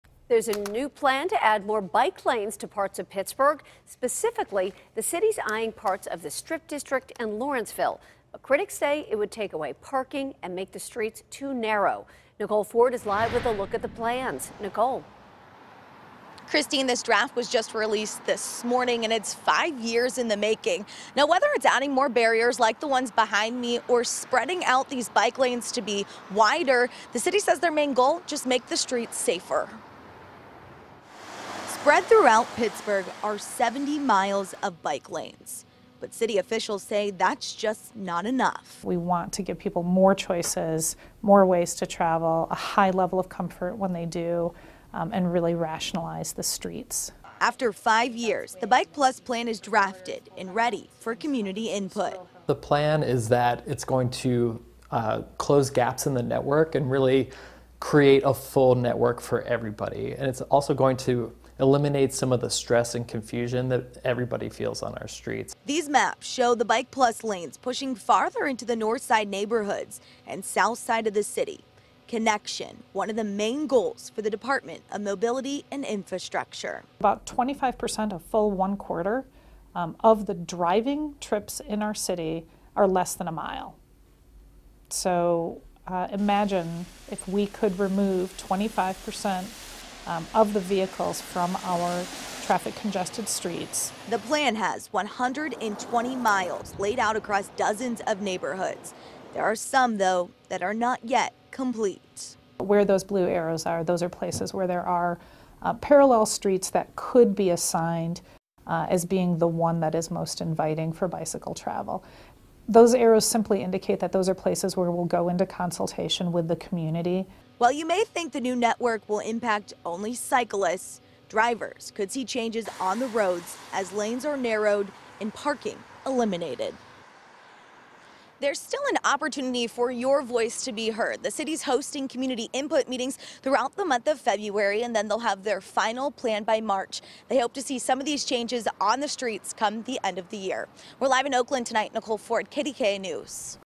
Pittsburgh Unveils Draft Of City's Master Bike Plan
There's a new plan to add more bike lanes to parts of Pittsburgh; KDKA's Nicole Ford reports.